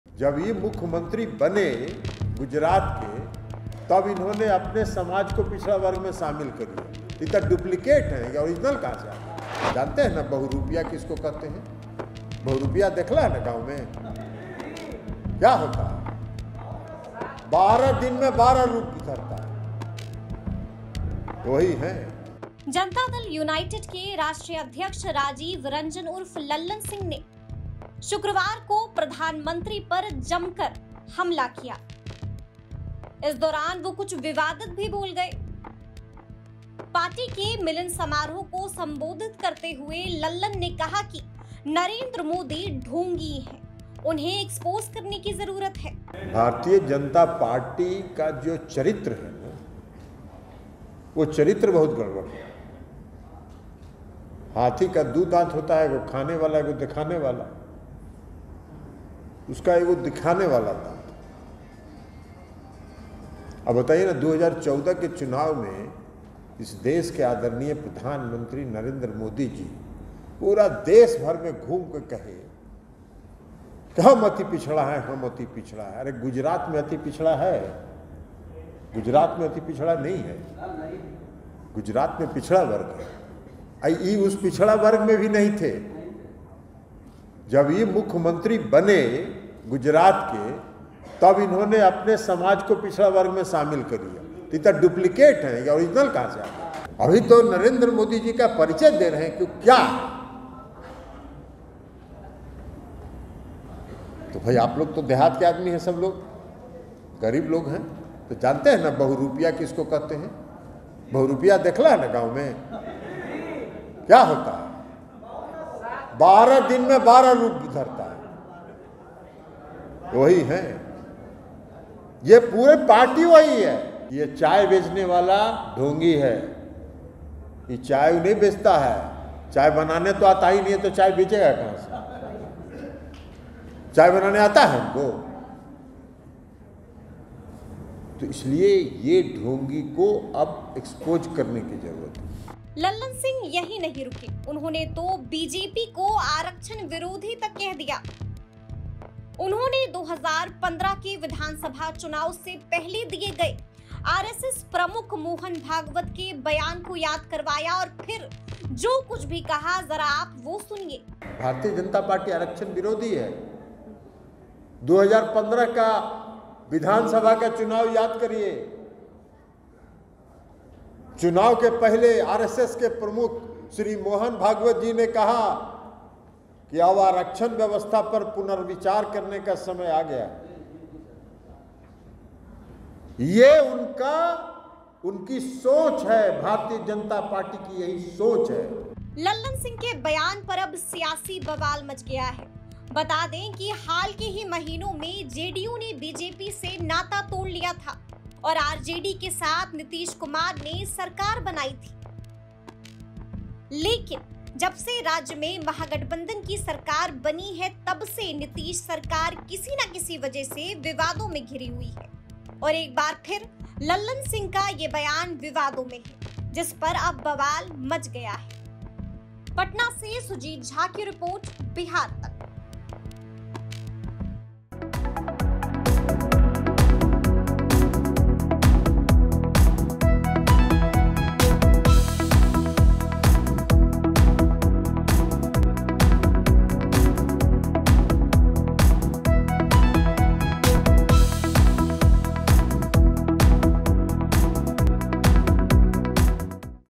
Lalan Singh ने PM मोदी का किया अपमान, जानिए ऐसा क्या कहा जिससे मच गया बवाल!
जेडीयू नेता व पार्टी के राष्ट्रीय अध्यक्ष ललन सिंह पीएम मोदी पर बड़ा हमला बोला है. शुक्रवार को एक कार्यक्रम को संबोधित करते हुए उन्होंने प्रधानमंत्री नरेन्द्र मोदी को डुप्लीकेट पिछड़ा बताया हैं. ललन सिंह ने कहा कि मोदी ओरिजनल नहीं बल्कि डुप्लीकेट ओबीसी हैं.